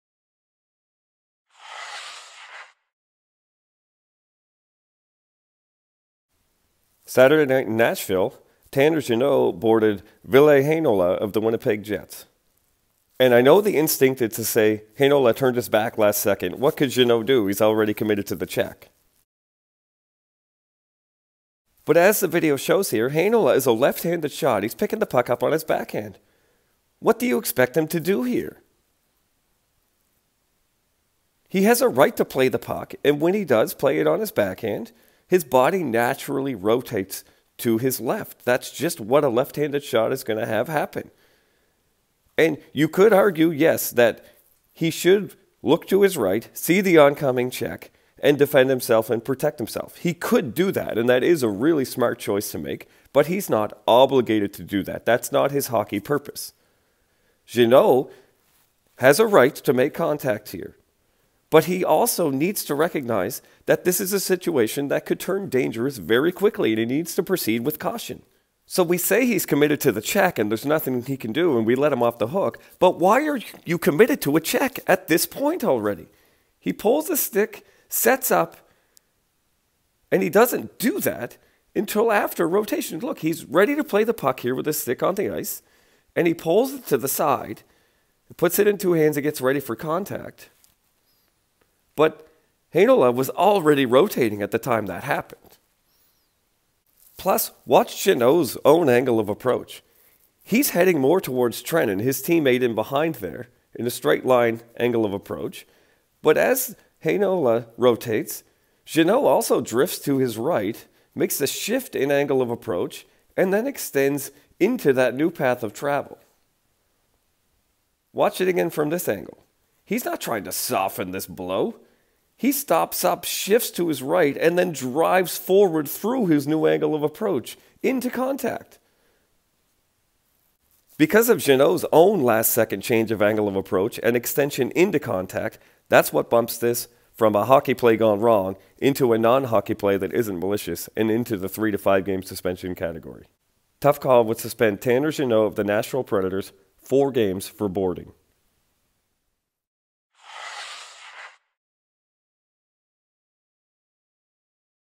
Tanner Jeannot boarding on Ville Heinola: Tough Call Suspension Recommendation
Tanner Jeannot of the Nashville Predators got a minor penalty for this boarding on Ville Heinola of the Winnipeg Jets.